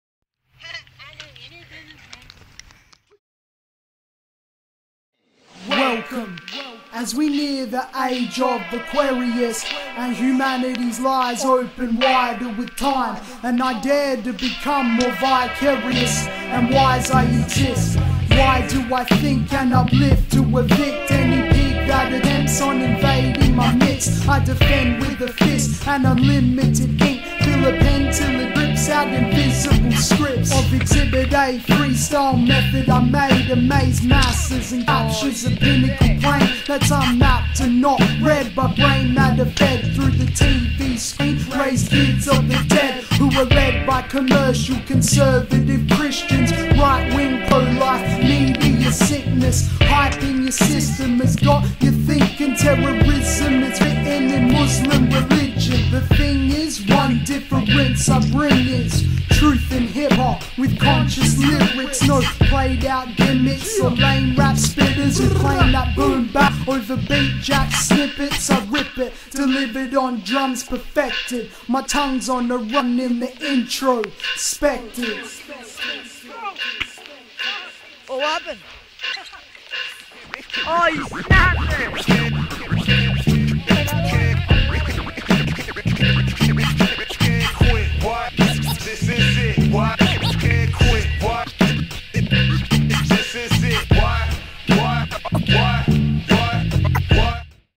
flagy scoot sesh
we where board so we made this vid not the best shit we can do.